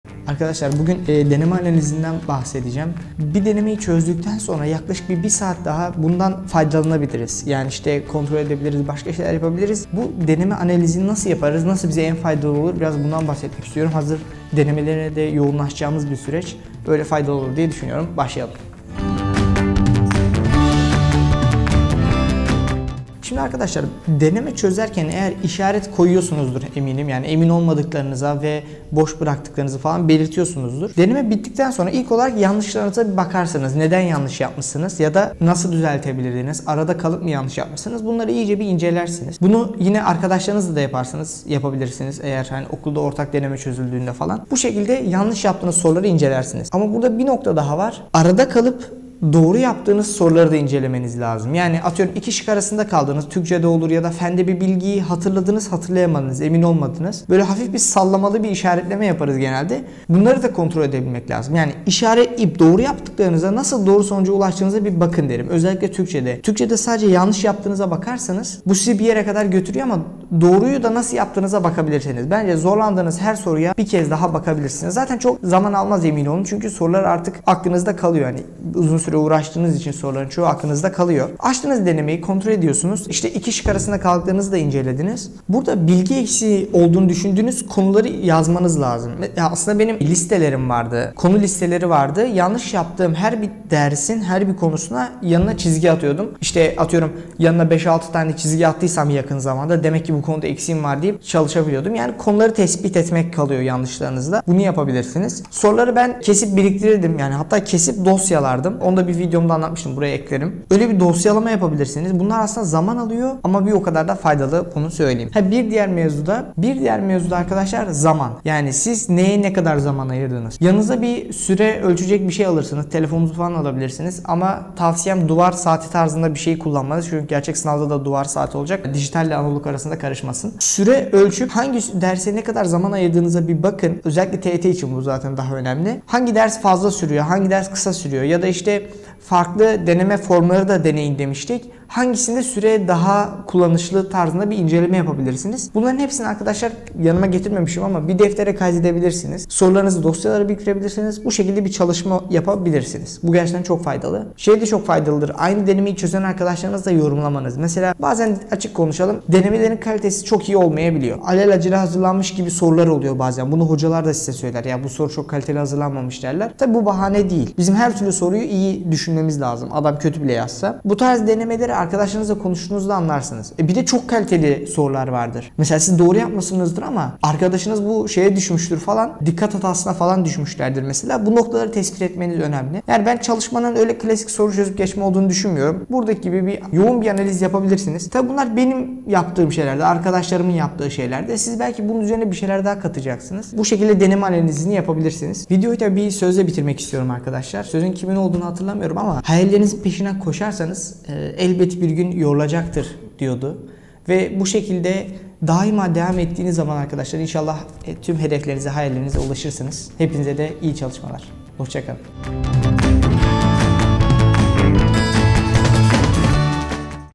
DENEME ANALİZİ YAPMANIN PÜF NOKTALARI | DENEME BİTTİKTEN SONRA ÇÖPE ATILIR MI?
Deneme çözüp sonrasında bunu çöpe atmak, doktorun verdiği reçeteyi çöpe atmak gibi olur. Peki denemeden sonra ne yapabiliriz. Bu videoda bunları ele aldık. Umarım Faydalı olur.

Bu arada instagram üzerinden bize sık gelen soruları post olarak cevaplıyoruz. Bir bakın derim. Belki sizin de merak ettikleriniz vardır. Sizin sorunuz varsa da yorum olarak sorabilirsiniz :)

Bu videoyu arkadaşlarınızla paylaşırsanız çok seviniriz. Beğenebilir ve görüşlerinizi yorumlarda belirtebilirsiniz:)
Konuşmacı: Ömer Faruk Doğru 2018 YKS Türkiye 12. si
Kameraman - Montaj : Oğuzhan Ballı